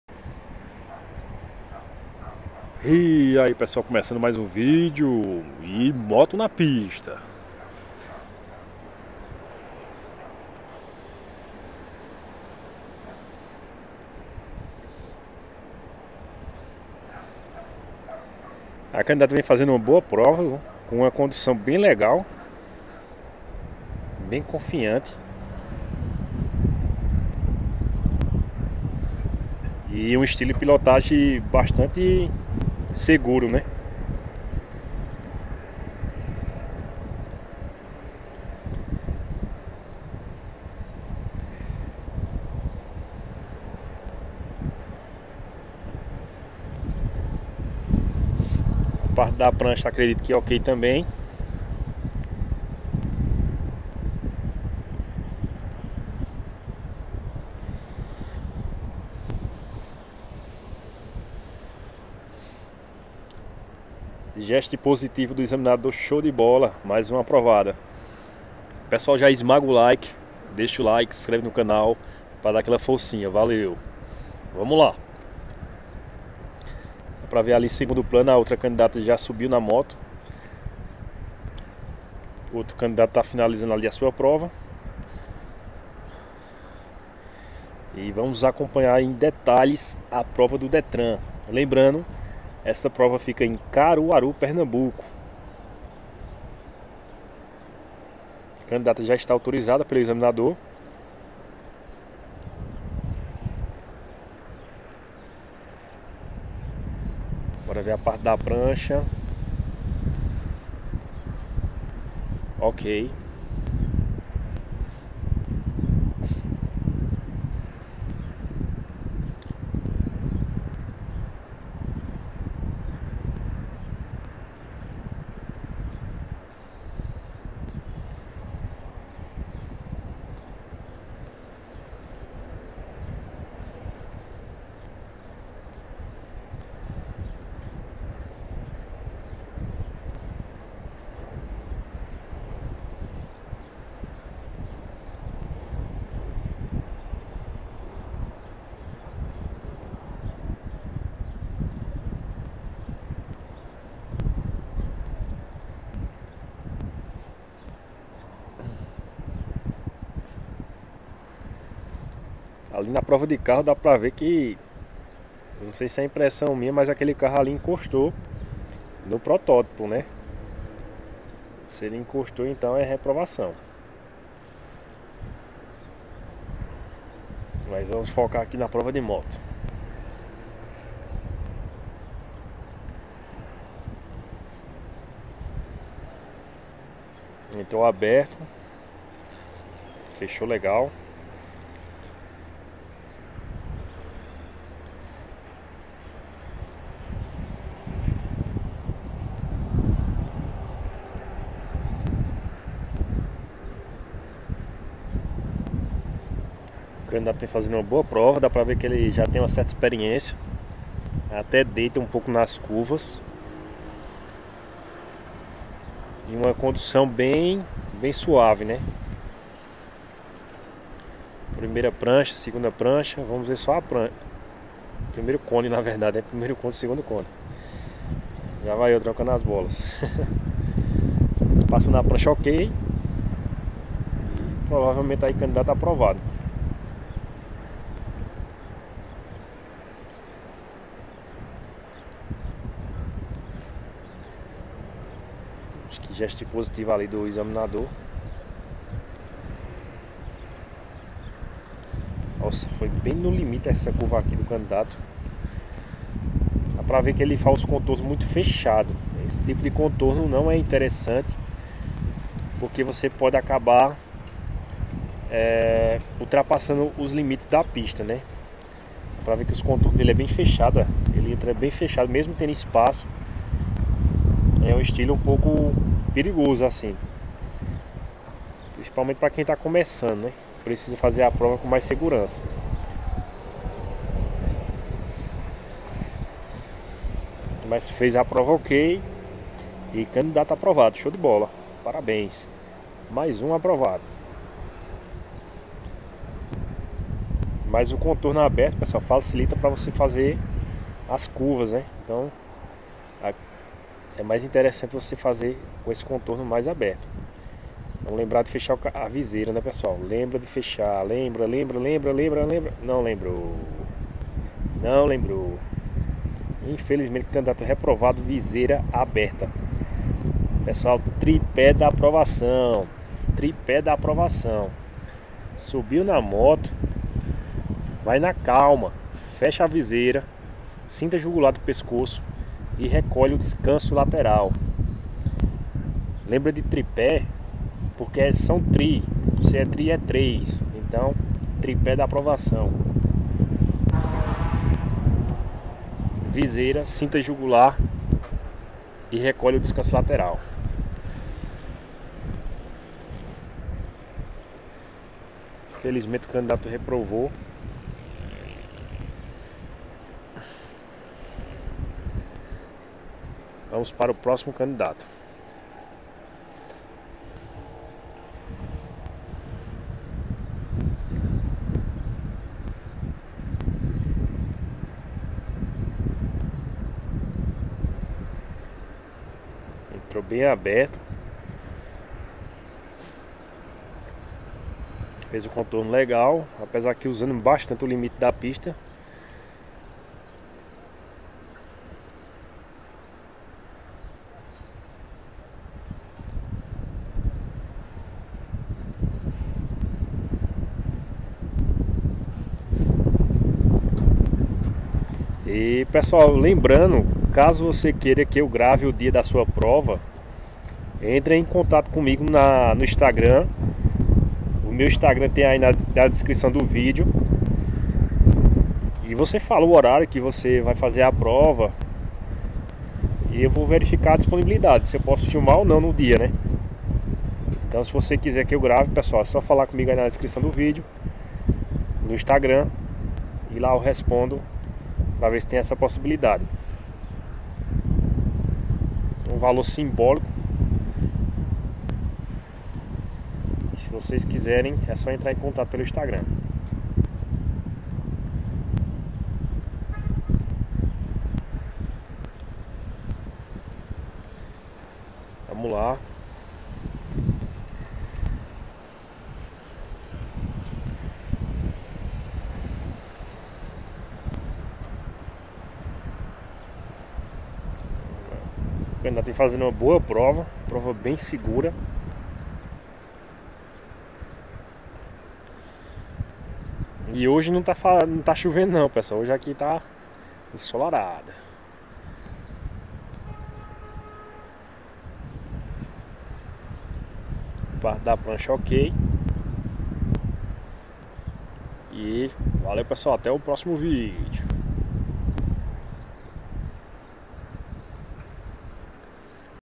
ESQUECEU VISEIRA ABERTA NO EXAME DE MOTO DO DETRAN #204
Exame prático de moto no DETRAN(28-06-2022) , confiram: ESQUECEU VISEIRA ABERTA NO EXAME DE MOTO DO DETRAN #204 . Este é último processo para se conseguir a Permissão Para Dirigir - PPD, que depois de um ano virará a tão sonhada Carteira Nacional de Habilitação - CNH.

LEMBRANDO QUE AS FILMAGENS FEITAS PELO O CANAL NÃO SERVEM PARA FIM DE RECURSO,CASO DESEJE ENTRAR COM RECURSO TEM AS PRÓPRIAS FILMAGENS DO CIRETRAN, VER MAIS NO PDF PÁGINA 14.
As penalidades que mais reprovam na prova de moto são: iniciar a prova sem estar com o capacete ajustado ou afivelado; deixar a viseira levantada; avançar sobre o meio-fio ou a indicação de parada obrigatória; bater em um ou mais cones de balizamento; colocar o(s) pé(s) no chão com a moto em movimento, descumprir o limite da pista,e, por último,  cair da moto ou provocar acidente durante o exame.